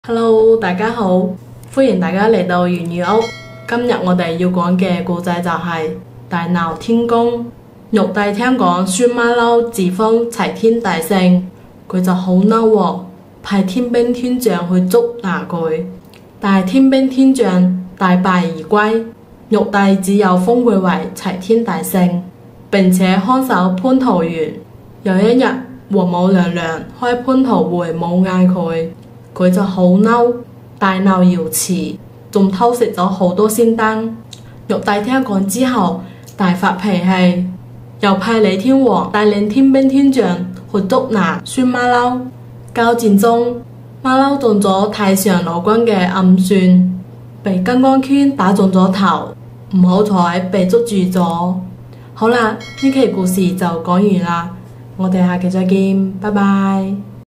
粵語古仔：大鬧天宮。粵語
今日我哋要分享嘅古仔就系
大鬧天宮
玉帝聽說孫猴子自封齊天大聖，
很生氣，派天兵天將來捉拿他。
可天兵天將大敗而歸。
玉帝只好封他為齊天大聖，
並看守蟠桃園。
有一天，王母娘娘開蟠桃會沒請他。
他生氣極了，大鬧瑤池，
還偷吃光了仙丹。
玉帝聽說後大發脾氣，
又派李天王帶領天兵天將去捉拿孫猴子。
交戰中，孫猴子遭了太上老君的暗算，
被金剛圈打中頭，不幸被捉。